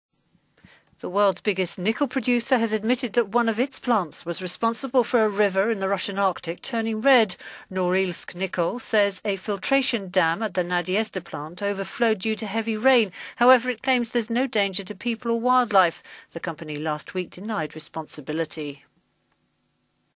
Nickel producer Norilsk admits plant spillage turned Russian river red
The world's biggest nickel producer has admitted that one of its  plants was responsible for a river in the Russian Arctic turning red.

Norilsk Nickel says a "filtration dam" at the Nadezhda plant over flowed due to heavy rain. However, it claims there is no danger to people or wildlife.

The company last week denied any responsibility when Greenpeace Russia provided pictures from the site, showing river banks covered in crimson slime.

The environmentalist group says it is too early to judge t…